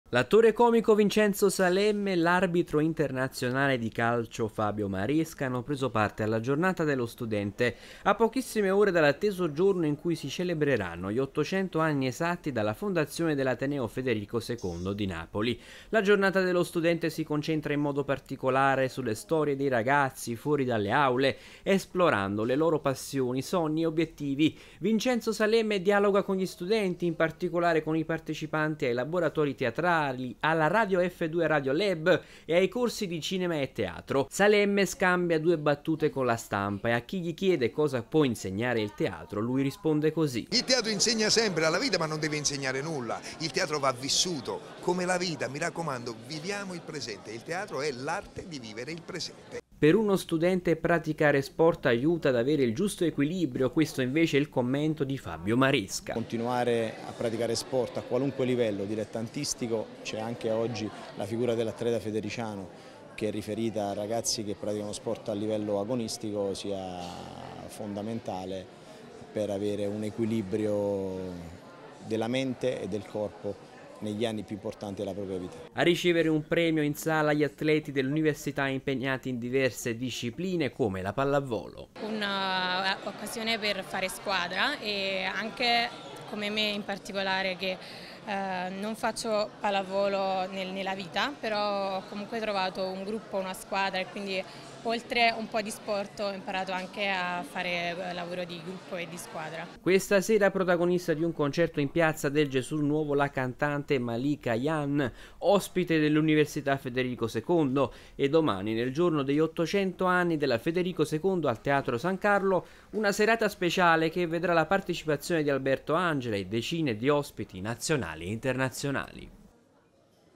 Università Federico II, Salemme e Maresca per gli 800 anni dell'Ateneo
L’attore e regista Vincenzo Salemme e l’arbitro Fabio Maresca incontrano gli studenti della Federico secondo nell’ambito dei festeggiamenti per gli 800 anni dalla nascita dell’ateneo.